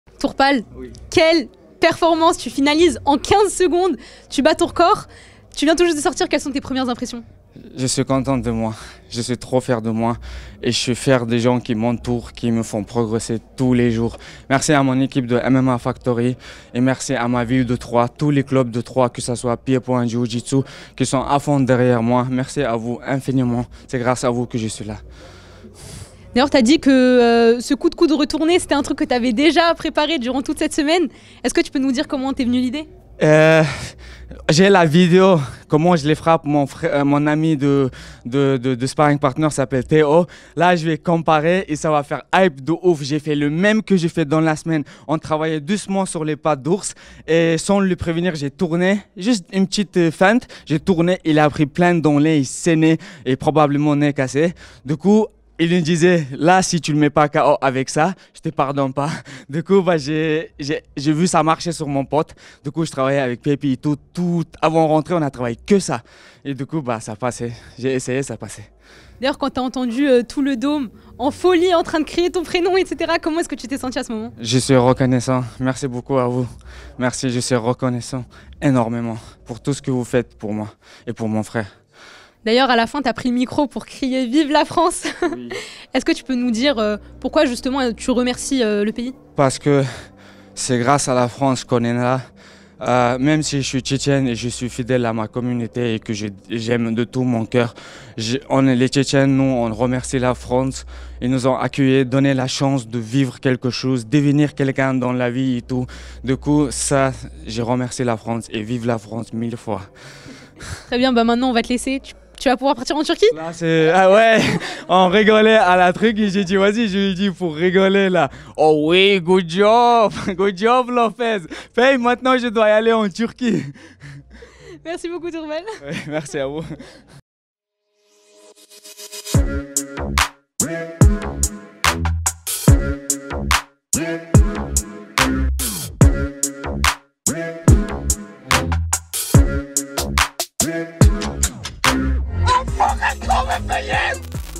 Turpal Younousov : "Fernand Lopez oh oui, good job!" | ARES 12
Turpal Younousov s'est imposé par soumission éclaire à ARES 12.

Lien vers le discord pour ne plus être solo pendant les fight nights

Soundcloud, Deezer, Apple Podcast, Spotify, le podcast est disponible sur toutes les plateformes audio

SUIVEZ-NOUS !